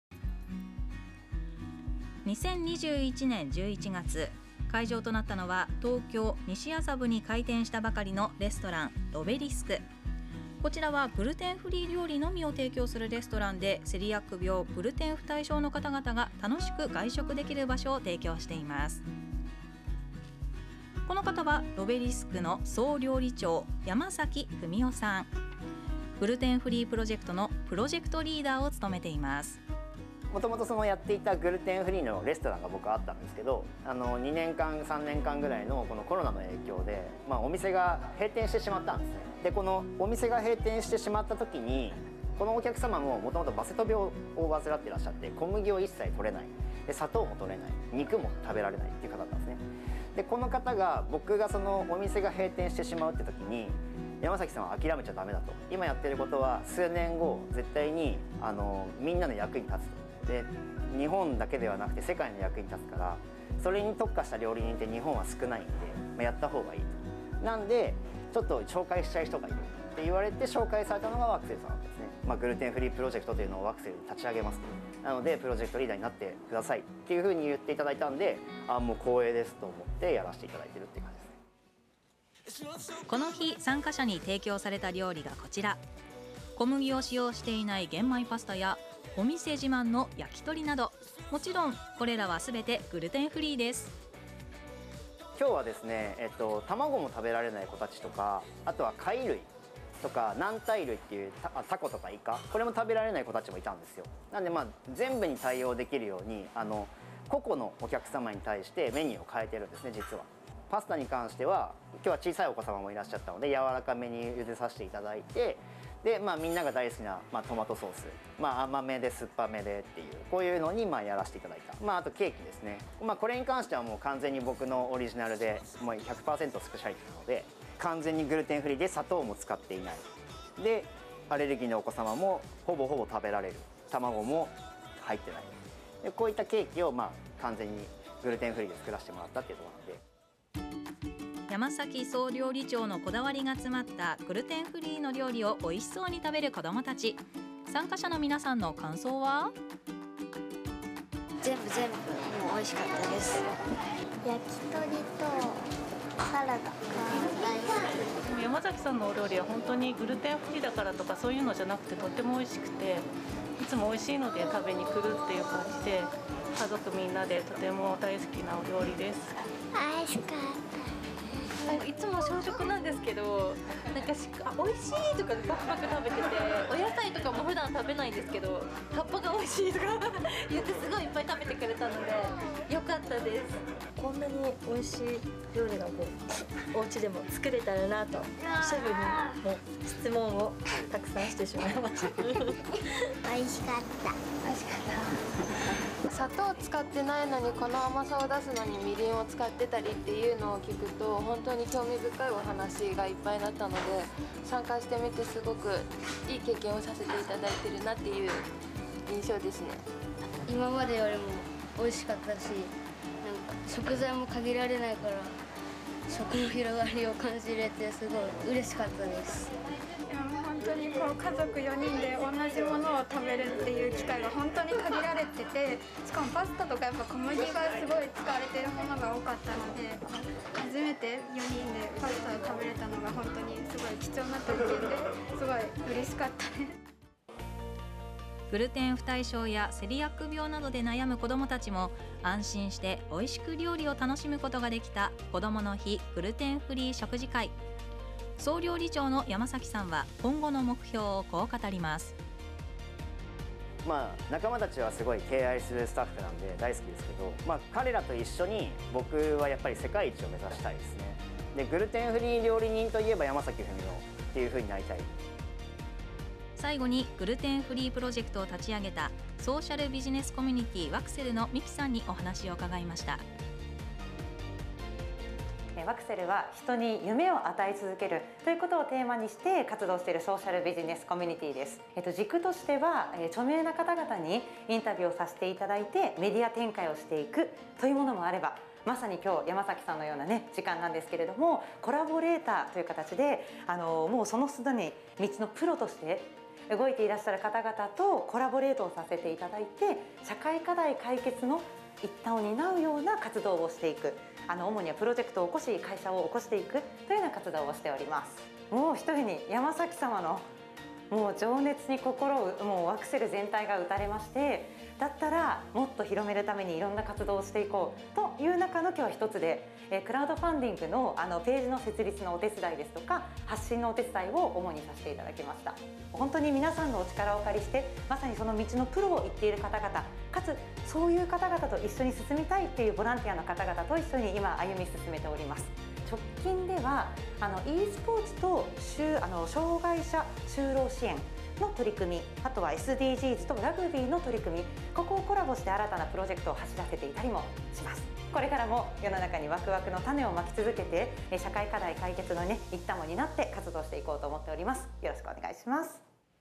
おいしいグルテンフリー料理を子どもたちにワクセル グルテンフリープロジェクト
ロベリスク。の山崎史雄総料理長がプロジェクトリーダーを務める「ワクセル　グルテンフリープロジェクト」
子どもの日グルテンフリー食事会の様子や参加者の感想をご紹介します。

ロベリスク。
※完全予約制